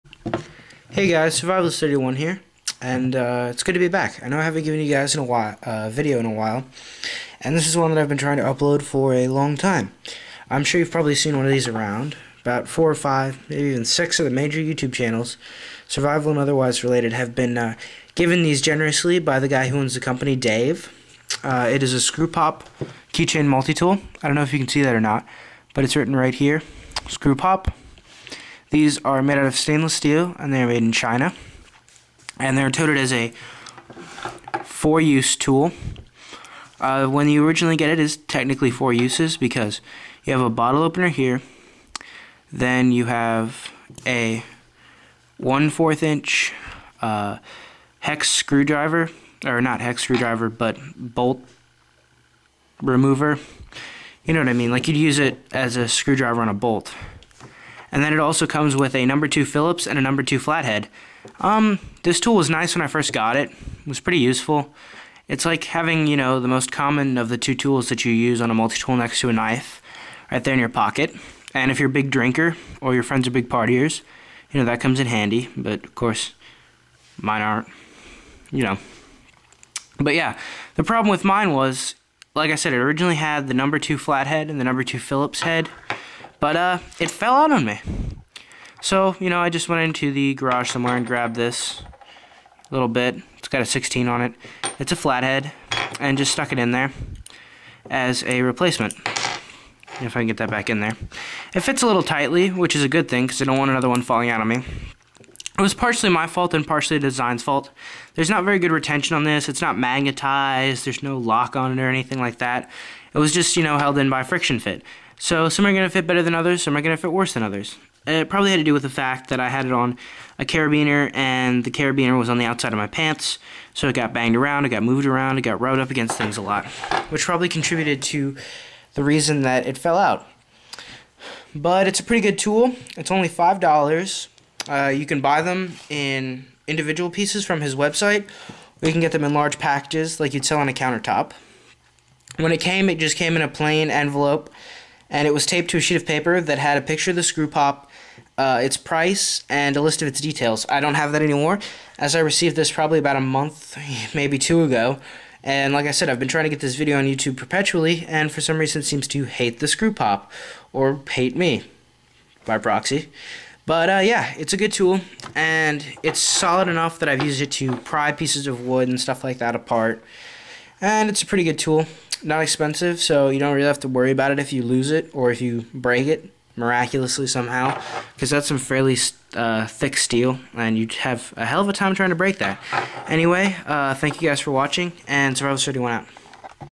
Screw pop review
This is my review of the screwpop key chain tool. Sorry the camera is shaky had a little trouble with the table i was filming on shacking. And sorry the owner of screw pop's name is brett sorry brett